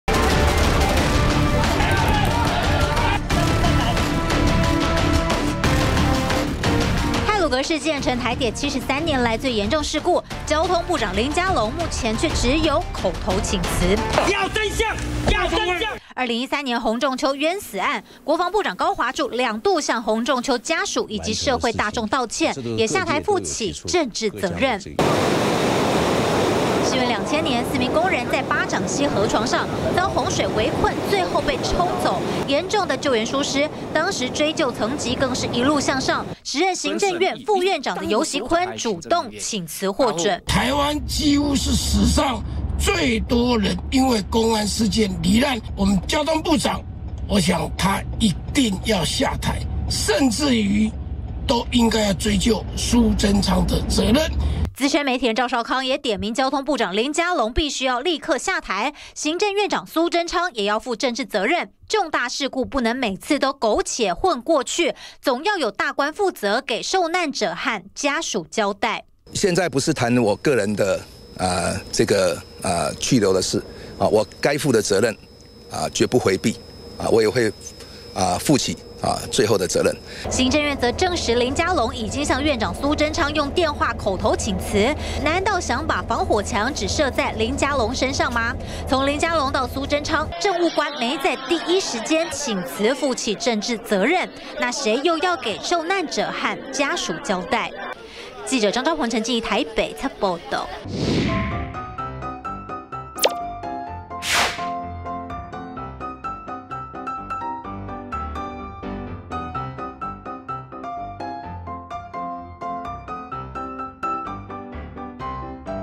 【太魯閣號出軌】太魯閣號究責只到林佳龍? 昔八掌溪事件副閣揆下台@中天新聞CtiNews 20210405
台鐵重大事故，目前卻只有交通部長林佳龍口頭請辭，難道是想把防火牆就設在林佳龍身上嗎。但回顧八掌溪事件，當時追究層級一路向上，到時任行政院的副院長游錫。資深媒體人趙少康就怒轟，不只林佳龍要下台，蘇貞昌也要負起責任。